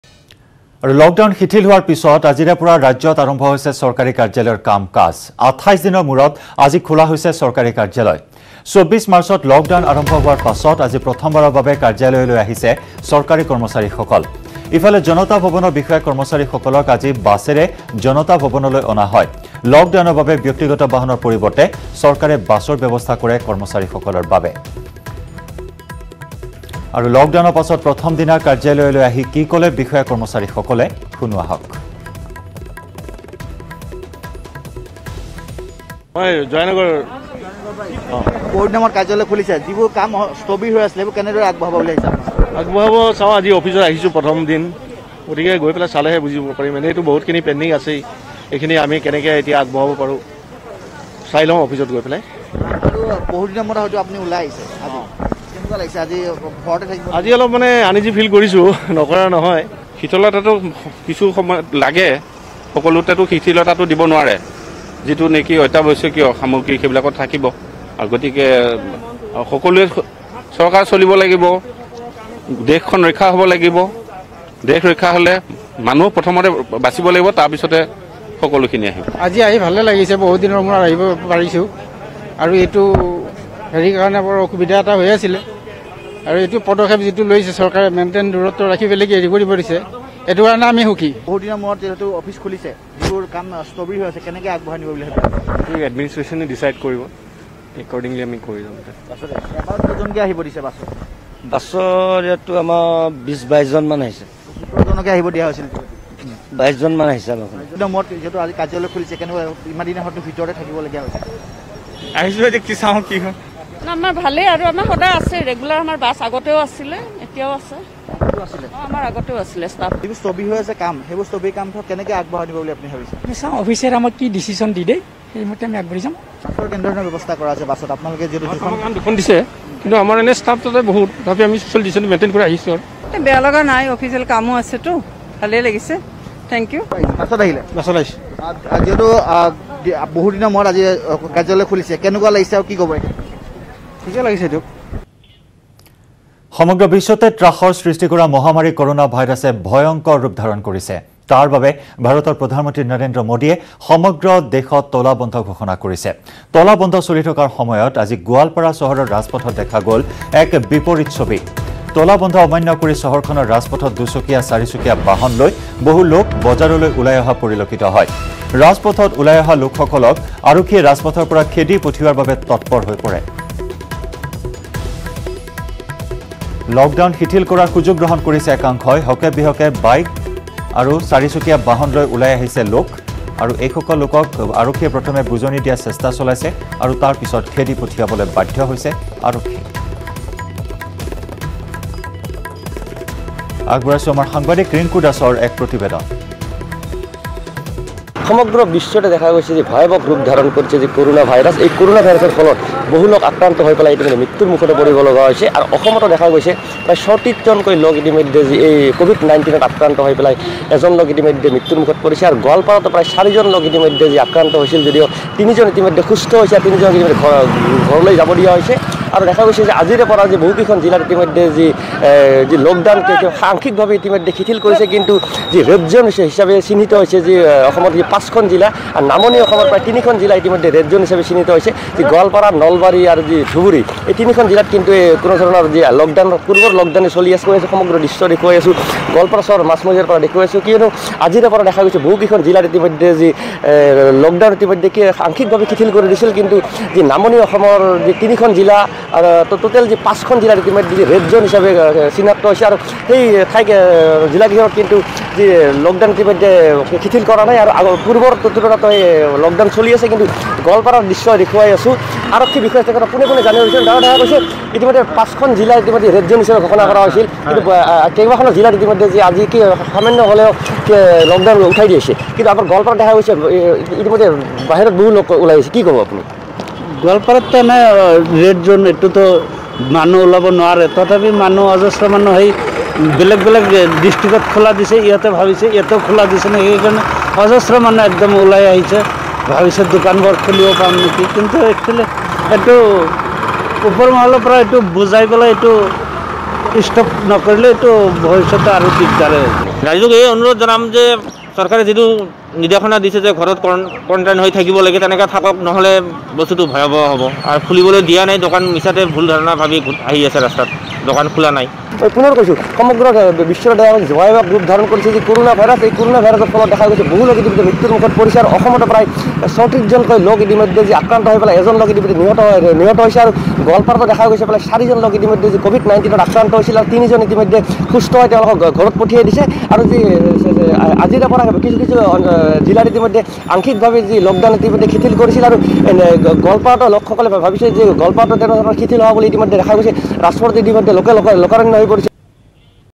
Lockdownৰ পিছত ৰাজ্য়ত আৰম্ভ হৈছে চৰকাৰী কাৰ্যালয় সমূহ
২৮ দিনৰ মূৰত আজি খোলা হৈছে চৰকাৰী কাৰ্যালয় সমূহ । জনতা ভৱন কৰ্মচাৰী সকলক বাছেৰে অনা হ'য় কাৰ্যালয়লৈ ।